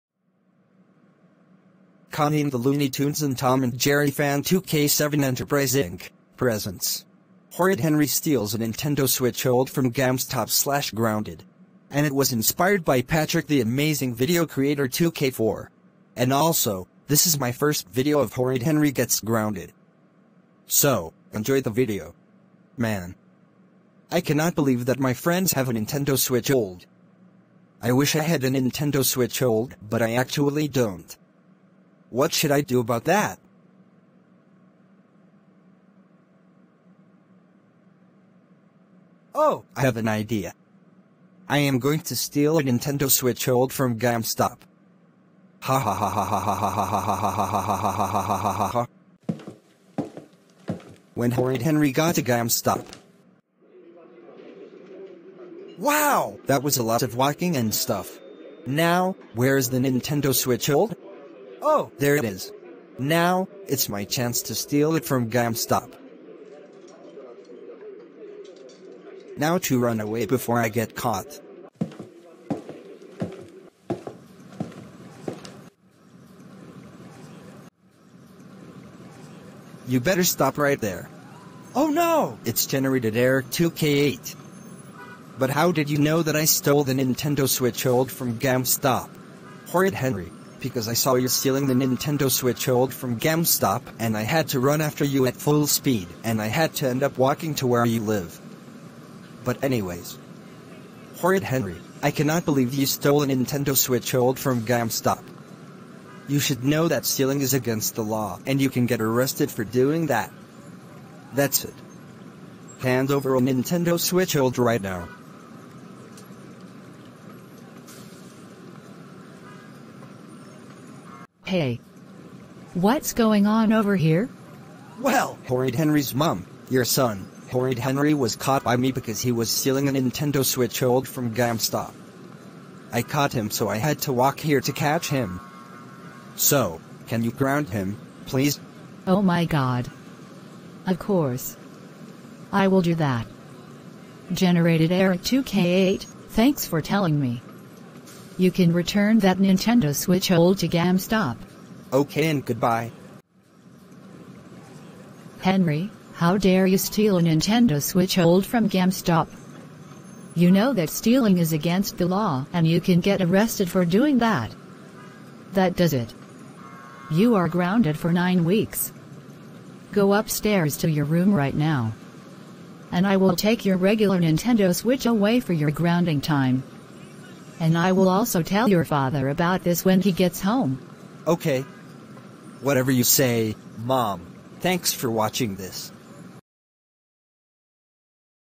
Horrid Henry steals a Nintendo Switch OLED from GameStop/grounded (Ft.
This is my first video of Horrid Henry gets grounded.

Inspired by: @PatrickTheVideoCreator62404Sr